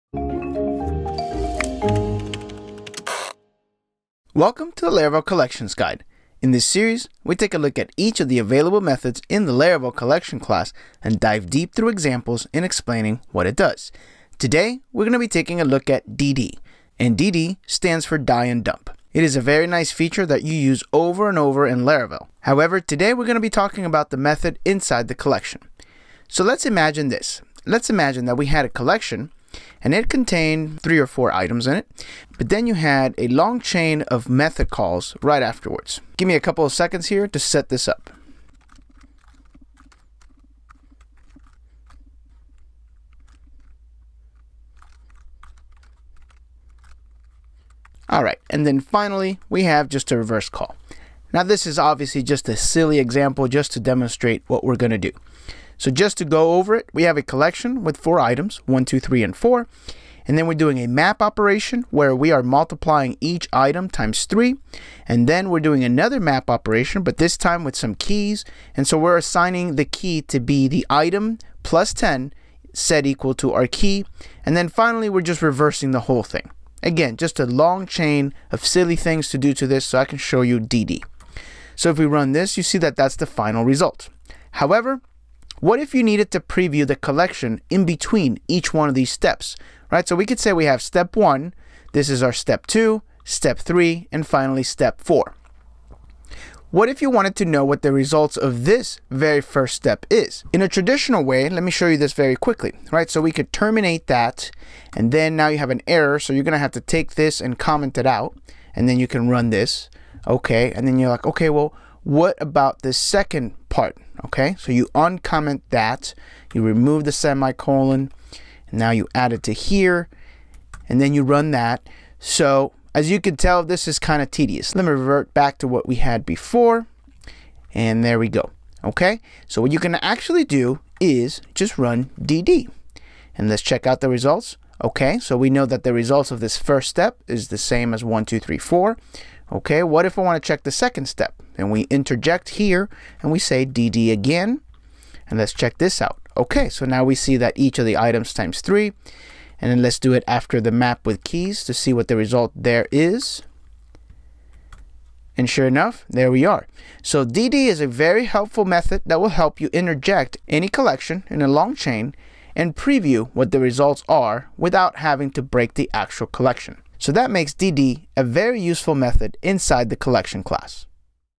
dd | Laravel Collections
When it comes to a long chain of method calls and transformations, sometimes, you may need to preview each step. In today's lesson, we talk about the dd() method and it does, just that.

In this series, we take a look at each of the available methods in the Laravel Collection class and dive deep through examples in explaining what it does.